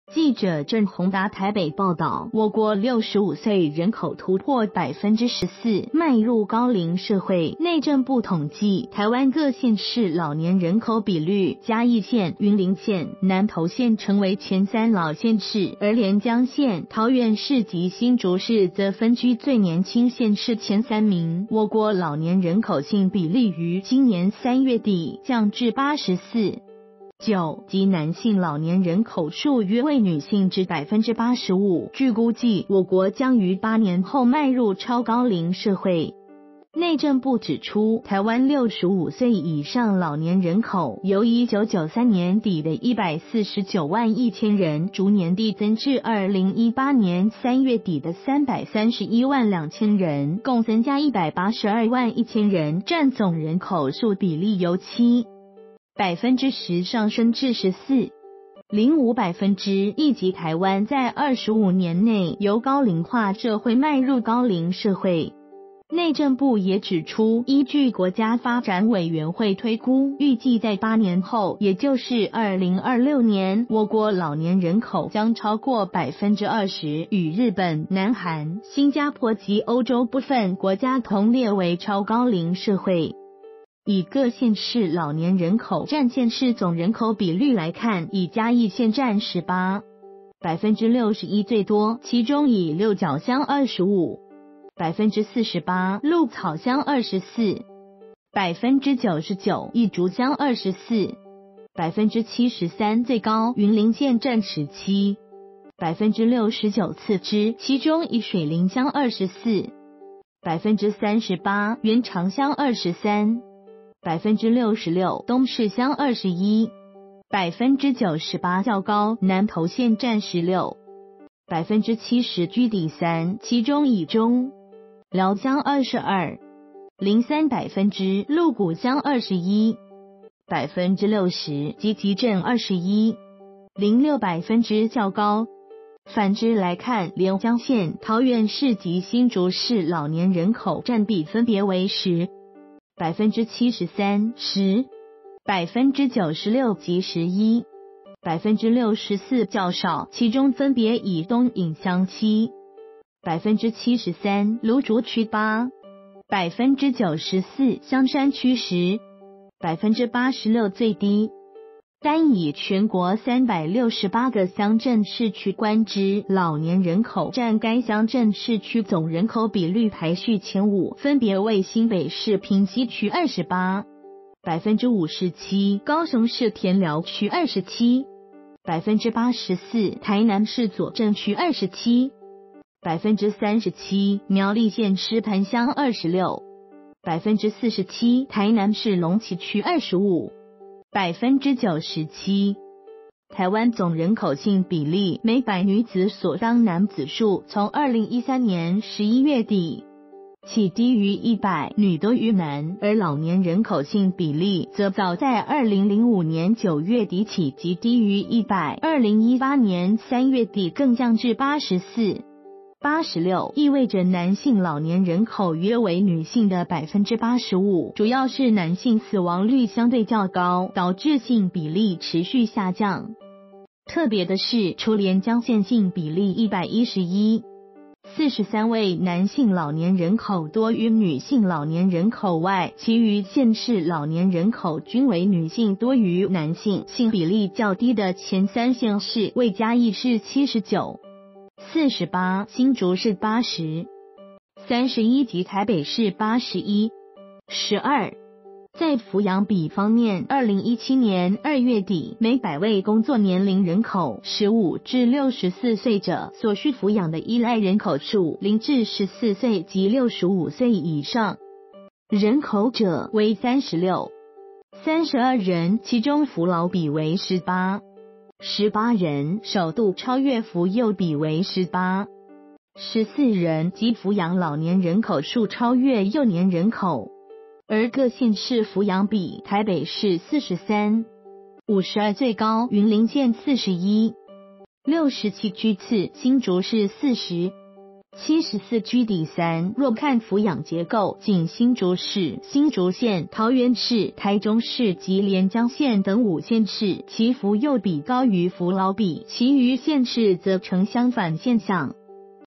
台灣邁入「高齡社會」 8年後達「超高齡」
〔記者鄭鴻達／台北報導〕我國65歲人口突破14%，邁入高齡社會。內政部統計，台灣各縣市老年人口比率，嘉義縣、雲林縣、南投縣成為前3老縣市；而連江縣、桃園市及新竹市則分居最年輕縣市前3名；我國老年人口性比例於今年3月底，降至84.9（即男性老年人口數約為女性之85%）；據估計，我國將於8年後邁入「超高齡社會」。

發表評論評論如下。